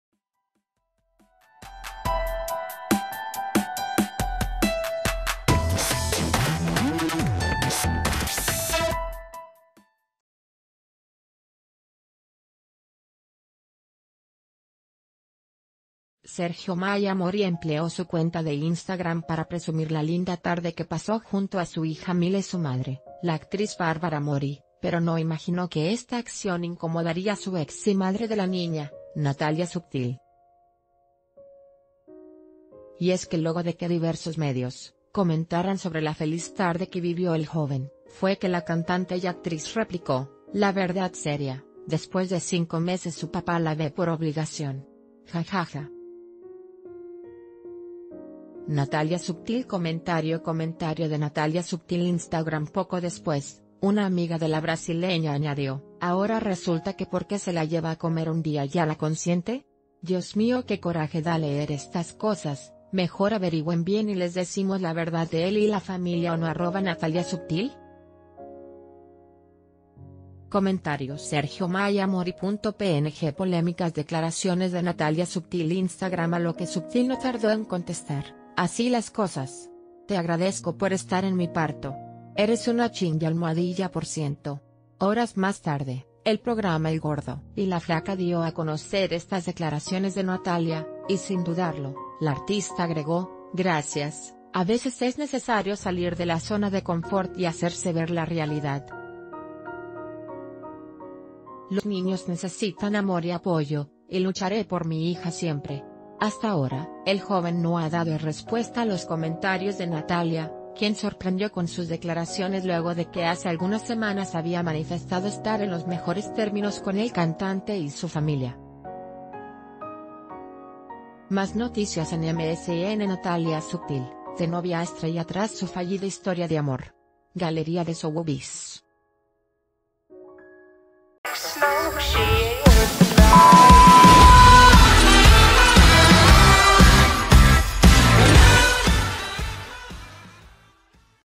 Natália Subtil hace polémica declaración sobre paternidad de Sergio Mayer Mori
Sergio Mayer Mori empleó su cuenta de Instagram para presumir la linda tarde que pasó junto a su hija Mila y su madre, la actriz Bárbara Mori, pero no imaginó que esta acción incomodaría a su ex y madre de la niña, Natália Subtil. Y es que luego de que diversos medios, comentaran sobre la feliz tard...